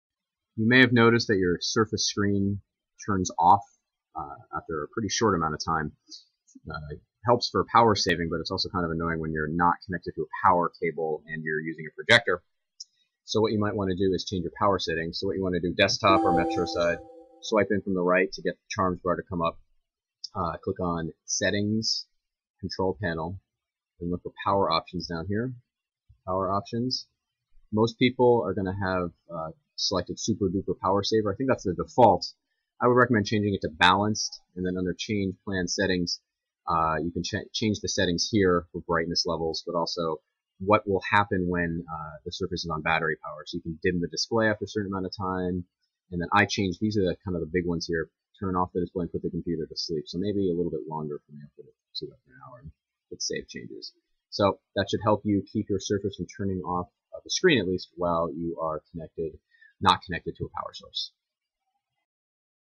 How to change the power settings on your Surface Pro
Keep the screen from turning off so quickly when you're running on battery power.